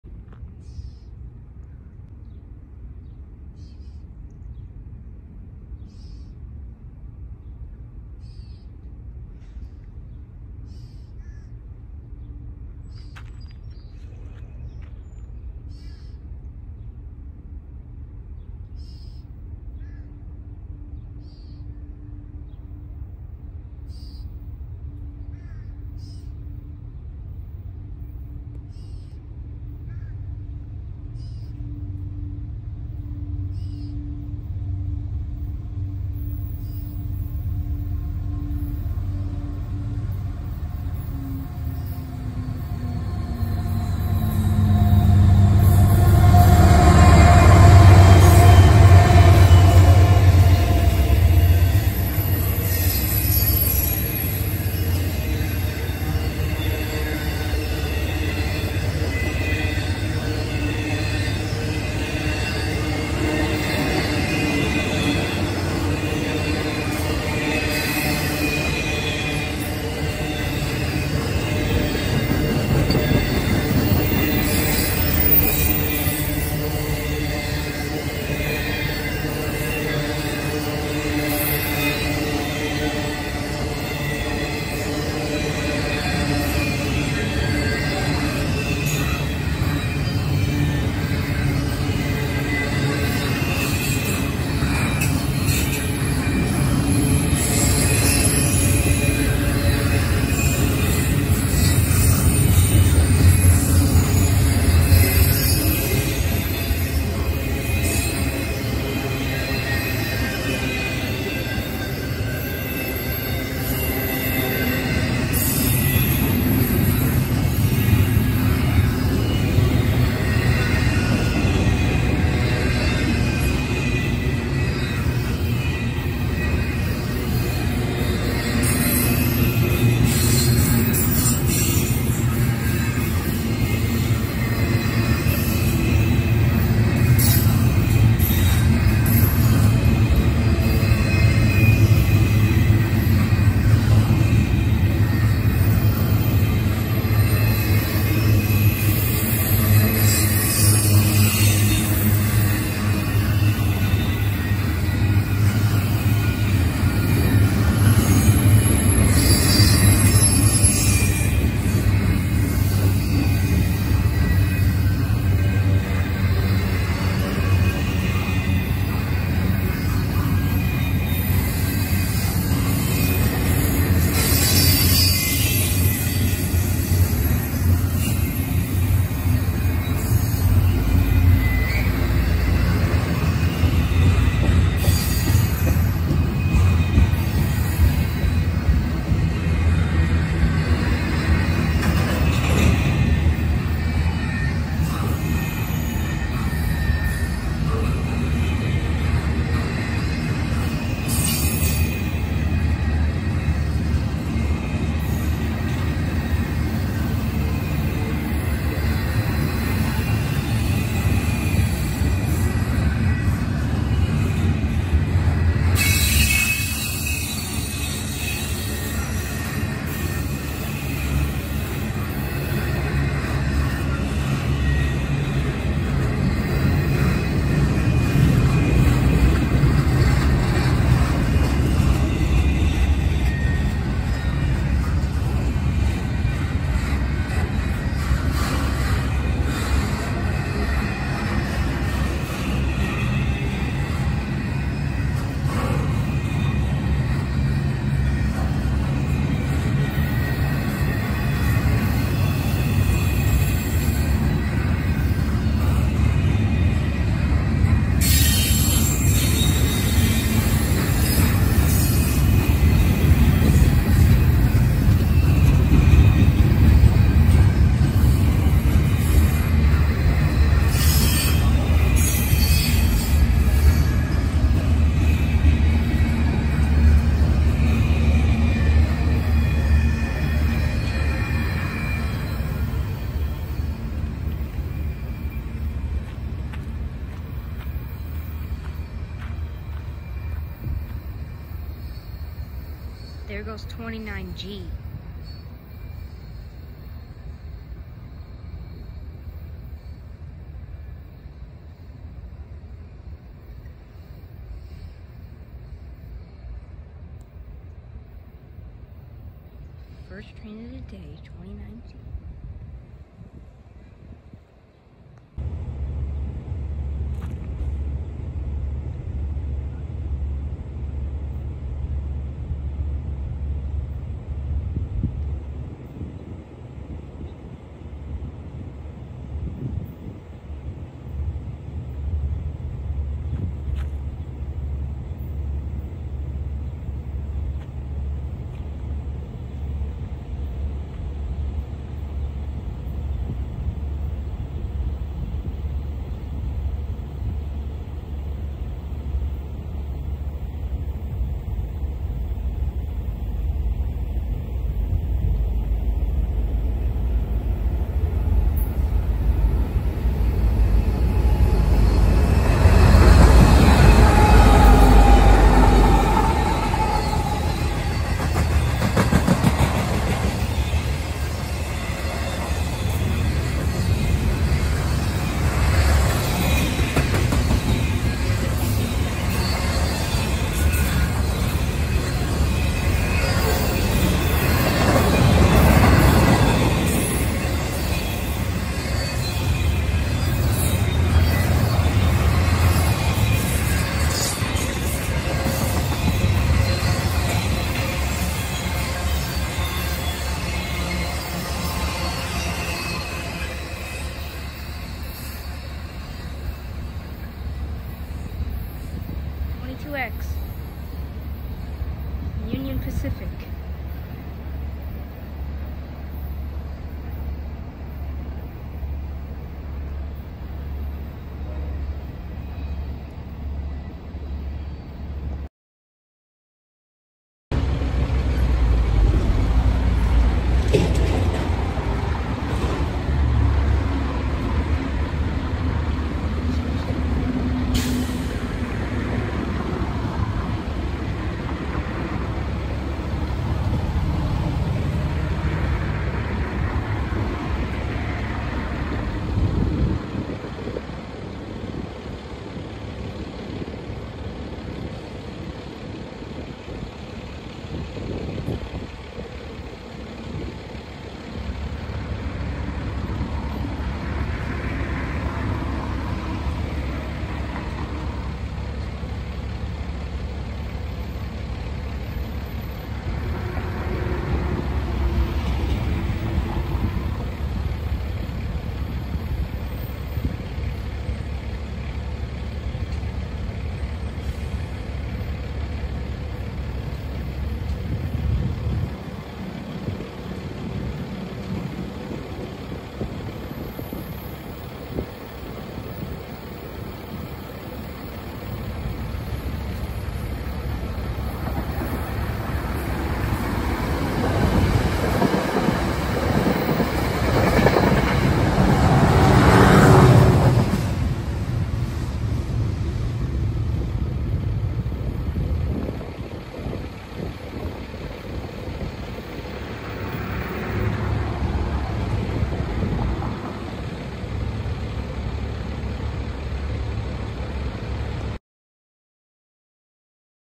Norfolk Southern & NJTransit Action in Central/North Jersey, Railfanning Manville-Paterson, NJ!!
Train 1.
NS 29G
Croxton, NJ - Chicago, IL (47th Street Yard)
Intermodel

Train 2.
NS 22X ft UP
Chicago, IL (47th Street Yard) - Croxton, NJ
Intermodel

Train 3.
NJT
Suffern, NY - Hoboken, NJ